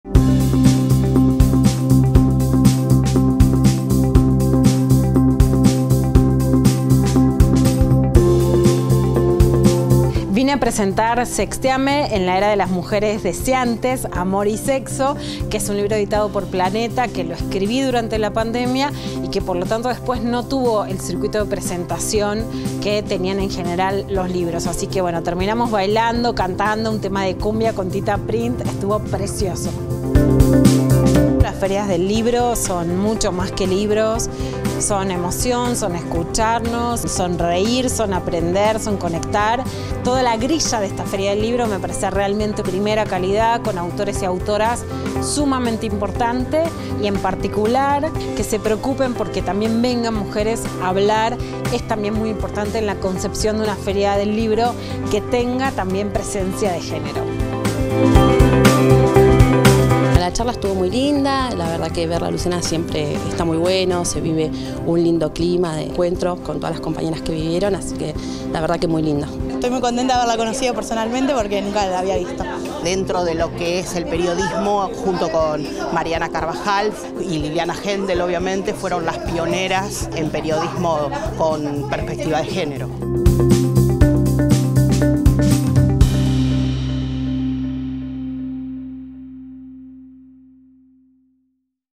LUCIANA PEKER SE PRESENTÓ EN LA FERIA DEL LIBRO
La escritora y periodista feminista visitó la 5° Edición de la Feria del Libro de Merlo y presentó su nuevo libro, “Sexteame, amor y sexo en la era de las mujeres deseantes” en la Sala Adán BuenosAyres  donde cientos de mujeres merlenses fueron partícipes.